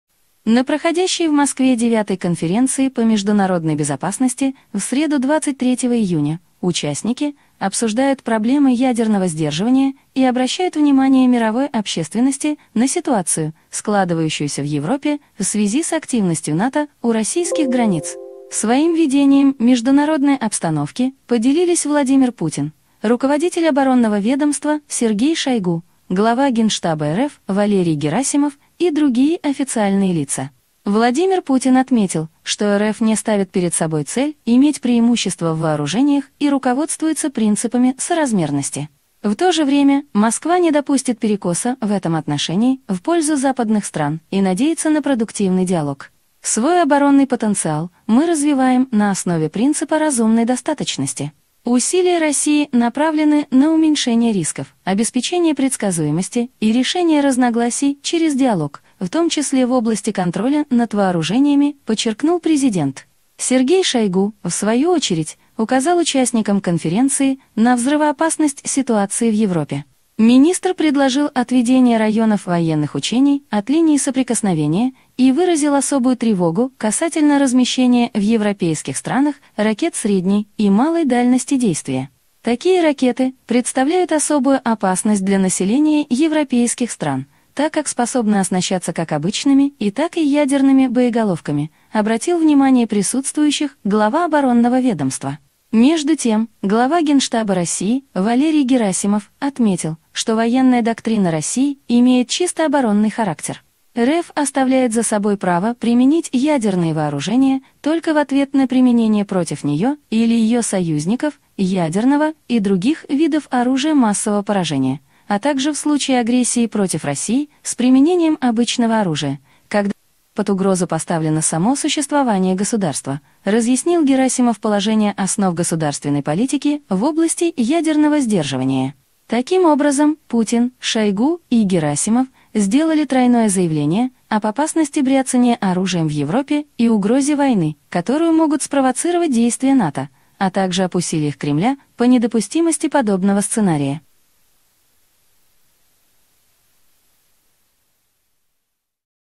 Путин, Шойгу и Герасимов рассказали о вероятности войны
На проходящей в Москве IX Конференции по международной безопасности в среду 23 июня участники обсуждают проблемы ядерного сдерживания и обращают внимание мировой общественности на ситуацию, складывающуюся в Европе в связи с активностью НАТО у российских границ. Своим видением международной обстановки поделились Владимир Путин, руководитель оборонного ведомства Сергей Шойгу, глава Генштаба РФ Валерий Герасимов и другие официальные лица.

Владимир Путин отметил, что РФ не ставит перед собой цель иметь преимущество в вооружениях и руководствуется принципами соразмерности. В то же время Москва не допустит перекоса в этом отношении в пользу западных стран и надеется на продуктивный диалог.

Свой оборонный потенциал мы развиваем на основе принципа разумной достаточности. Усилия России направлены на уменьшение рисков, обеспечение предсказуемости и решение разногласий через диалог, в том числе в области контроля над вооружениями

– подчеркнул президент.

Сергей Шойгу в свою очередь указал участникам Конференции на взрывоопасность ситуации в Европе. Министр предложил отведение районов военных учений от линии соприкосновения и выразил особую тревогу касательно размещения в европейских странах ракет средней и малой дальности действия.

Такие ракеты представляют особую опасность для населения европейских стран, так как способны оснащаться как обычными, и так и ядерными боеголовками

– обратил внимание присутствующих глава оборонного ведомства.

Между тем, глава Генштаба России Валерий Герасимов отметил, что военная доктрина России имеет чисто оборонный характер.

РФ оставляет за собой право применить ядерные вооружения только в ответ на применение против нее или ее союзников ядерного и других видов оружия массового поражения, а также в случае агрессии против России с применением обычного оружия, когда под угрозу поставлено само существование государства

– разъяснил Герасимов положения Основ государственной политики в области ядерного сдерживания.

Таким образом, Путин, Шойгу и Герасимов сделали тройное заявление об опасности «бр+яцания оружием» в Европе и угрозе войны, которую могут спровоцировать действия НАТО, а также об усилиях Кремля по недопустимости подобного сценария.